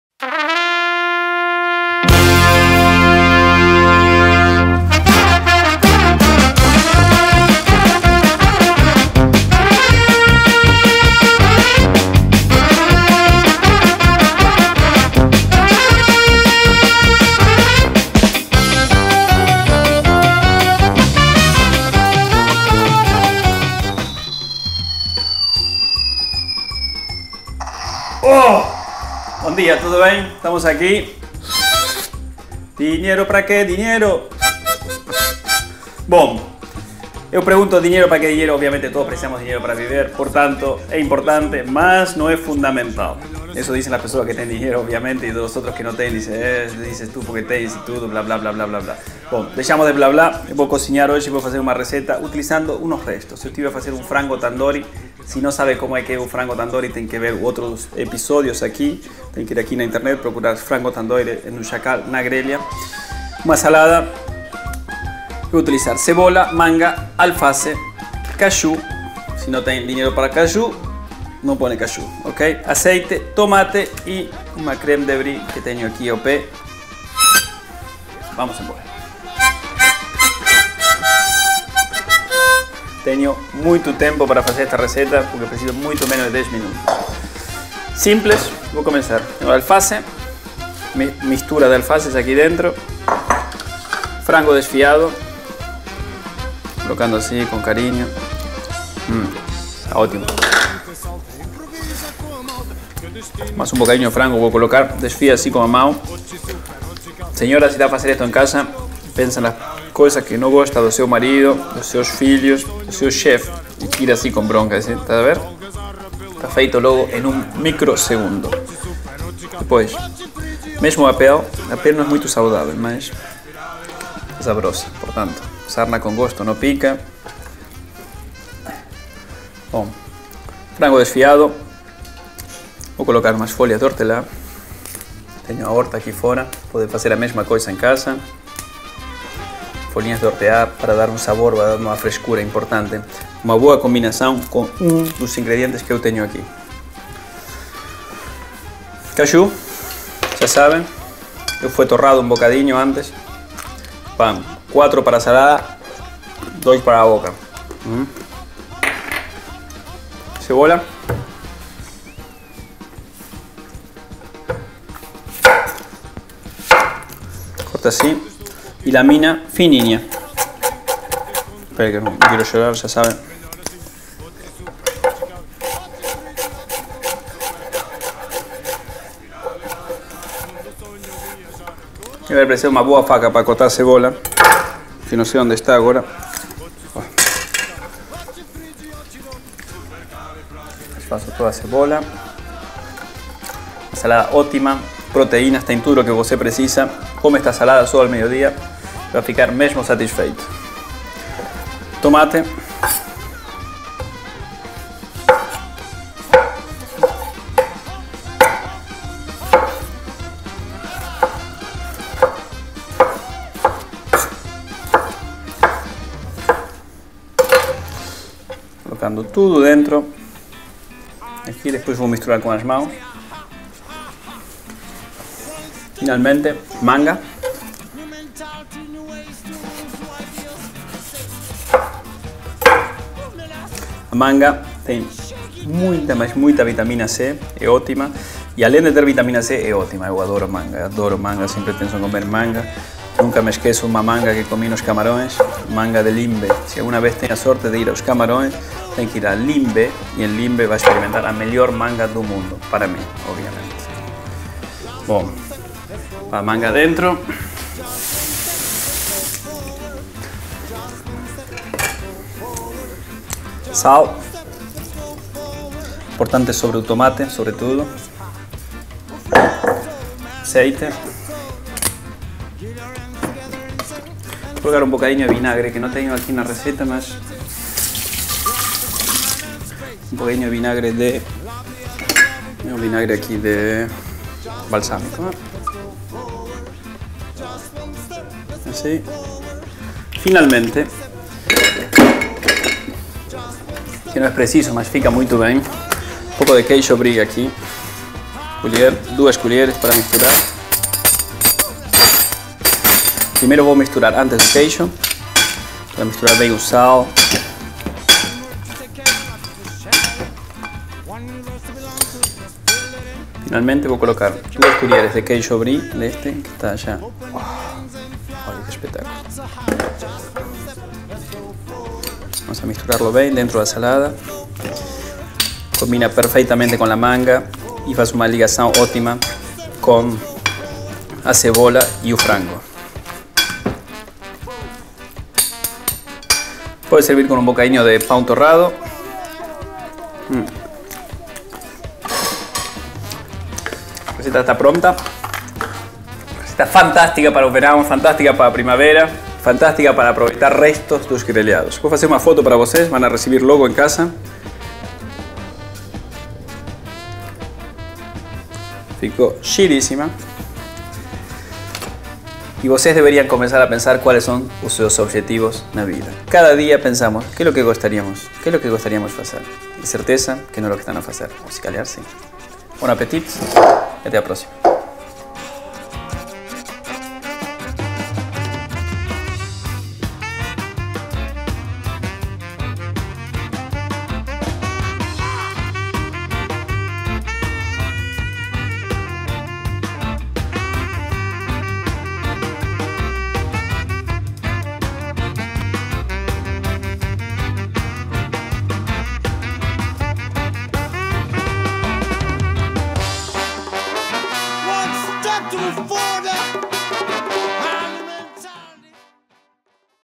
Salada Tandoori
Receita para a Troika do Chakall

Salada Tandoori. Custo aproximado:8,92 €

Ingredientes:

- 4 coxas de frango Tandoori
- 1 alface
- 1 tomate
- 20g de caju
- 1/2 cebola
- 1 manga
- hortelã q.b.
- 1 queijo Creme Brie
- sol e azeite q.b.
- vinagre balsâmico q.b.